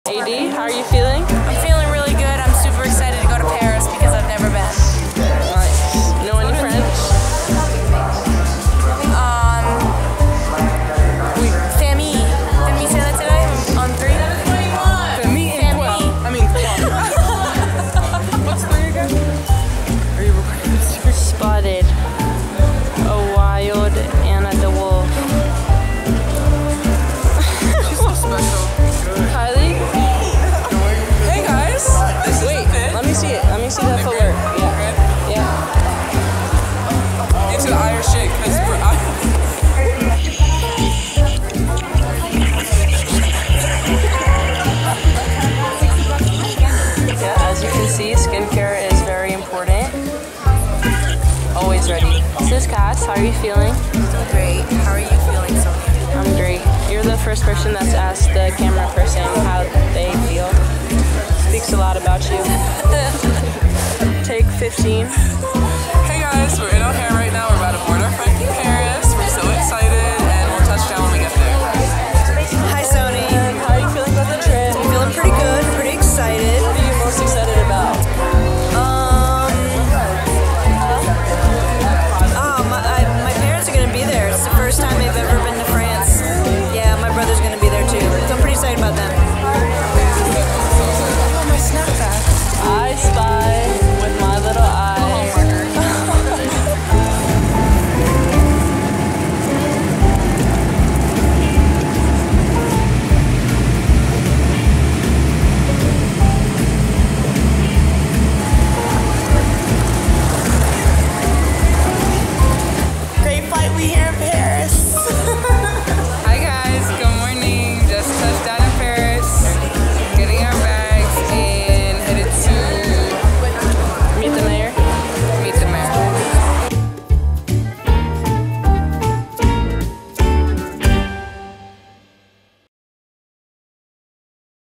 A little 𝑏𝑜𝑛 𝑣𝑜𝑦𝑎𝑔𝑒 vlog to kick off our trip | Notre Dame Women's Basketball
Notre Dame heads to Paris to play South Carolina in the first ever collegiate basketball game — men’s or women’s — played in the City of Lights.